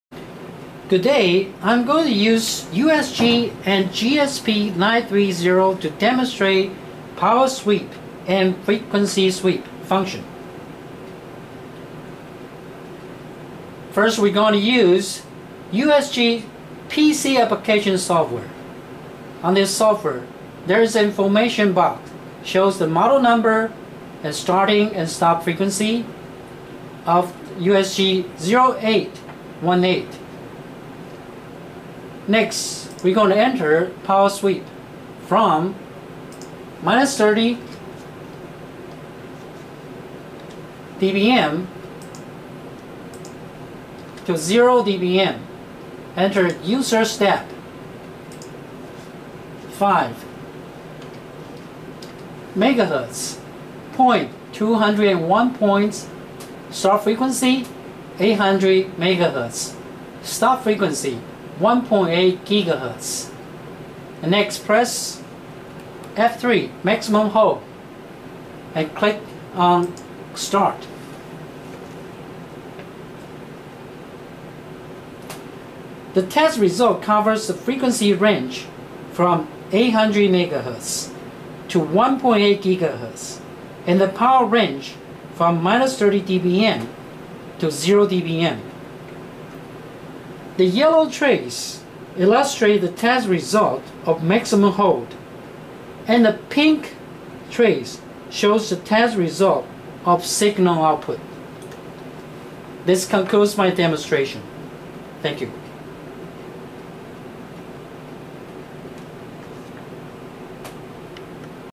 GW Instek USG RF Signal Generator PC Application Program Introduction
GW Instek USG RF Signal Generator PC Application Program based on JAVA Platform. It supports operating systems such as Windows 2000/XP/Vista/7/8, Linux & Mac OS X through the USB interface.